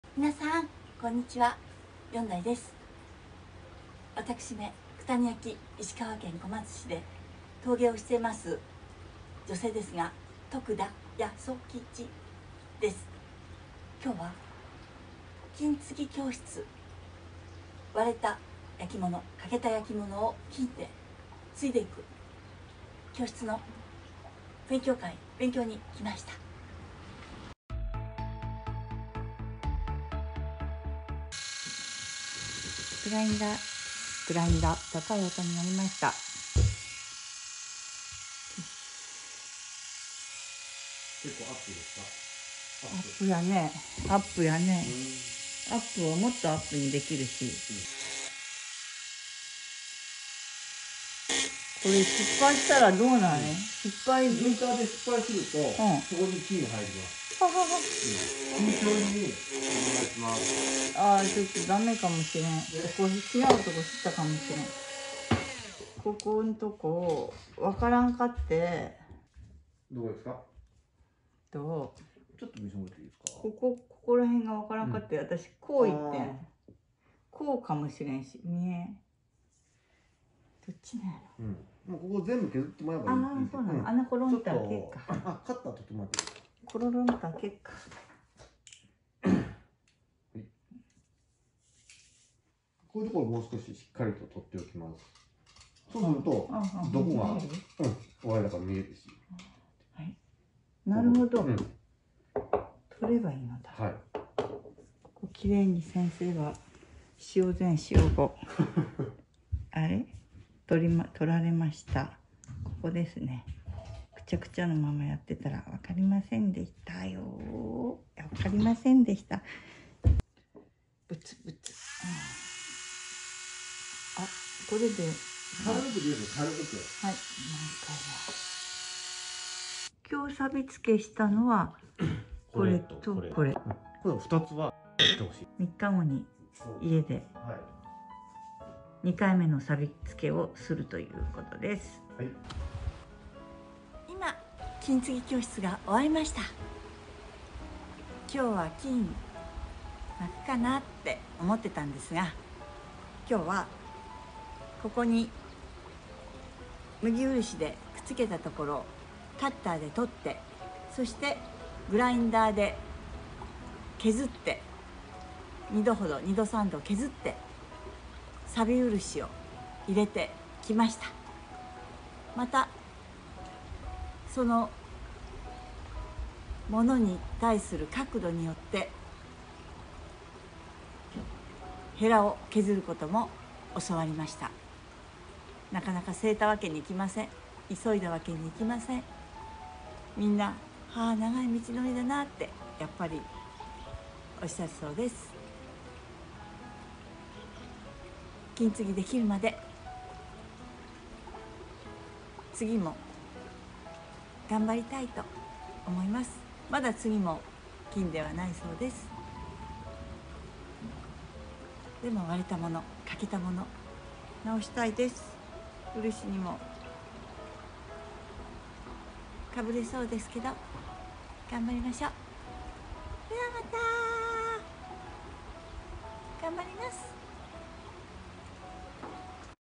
「割れたもの、かけた物、直したい‼️」金継ぎ教室行って来ました‼️道のりは長いけど もう少しです‼️夏までには、技術を習得したいです‼️
二週間ぶりの「金継ぎ教室」です❗「もうそろそろ、金まけるかな」なんて思って、三脚持って行ったのですが、なんのなんの😱😱😱めちゃめちゃ甘かったです🥶🥶🥶まず、カッターで漆を削る所から　もうダメ出し😭😭😭グラインダーも持って行った歯は、❓❓❓
（　＾ω＾）・・・全部使えず👽👽👽新品を分けてもらいました❗❗
ここから、グラインダーをかけたり、うるしを塗ったり・・・繰り返し地道な作業は続きます。その間、ヘラを削り直したり　乾いたうるしに水を入れて練ってもらい,
こしたり・・・さらに地道な作業は果てしない・・・
いつになったら、完成するのか❓❓❓
　　　　　今日で５回めだヨン

行く前に、ロクロ師と「買うたほうが、早い」と話してたっけ😉😁🤣
バブルの頃は、三代（父）なんか　「トラック８台半期で割れ物ほりに行っとった」と聞いている😲

とにかく、２時間みっちり作業すると　ぐったりするけど　ここまで来たらがんばるしかない❗❗
いつか、皆様のお宝を修復させて頂きたいと思います。それが、私の夢なのです💝